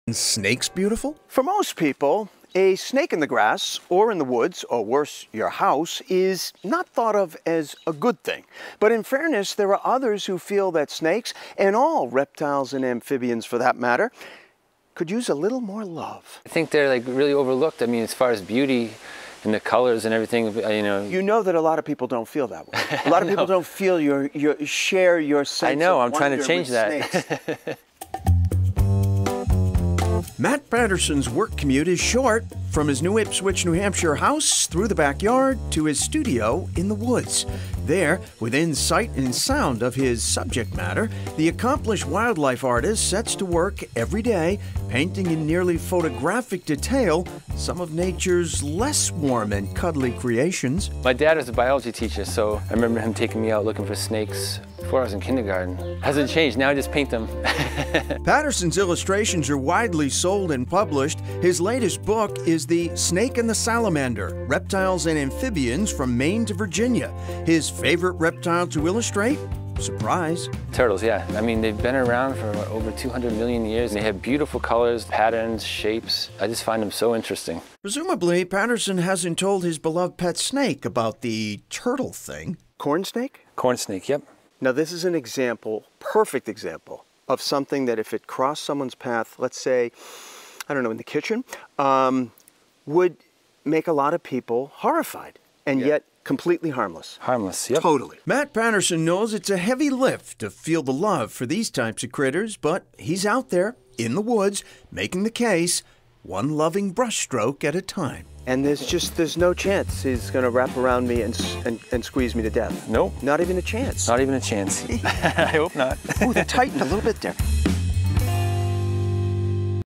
Local wildlife artist finds beauty in reptiles
Meet an accomplished wildlife artist on a mission to give snakes, frogs and other reptiles and amphibians a little more love.